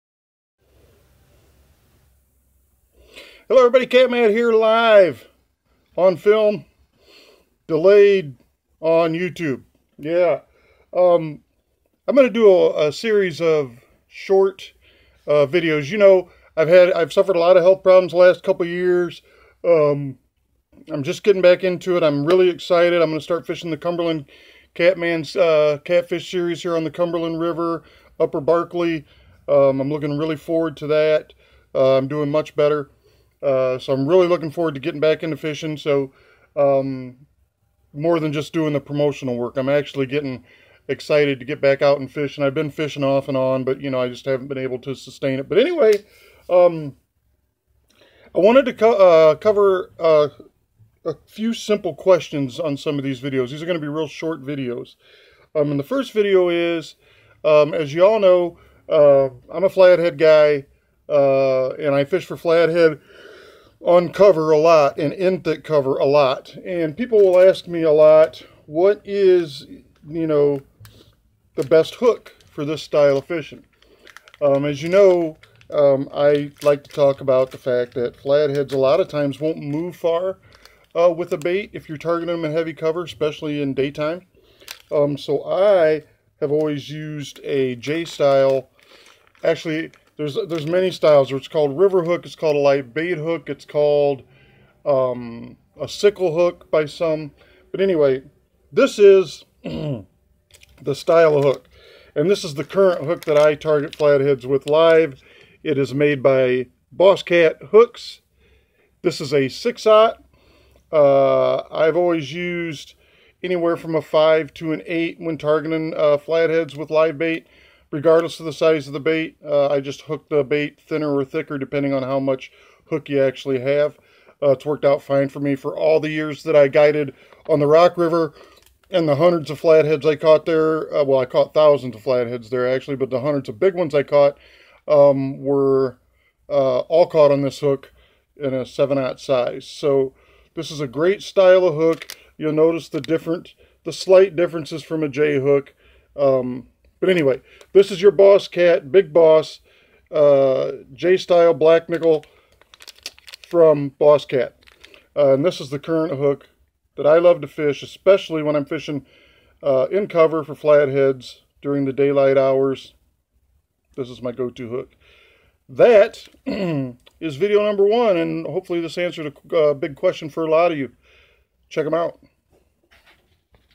Daytime Flathead hooks..
fishing for monster flathead catfish in heavy cover during daylight hours.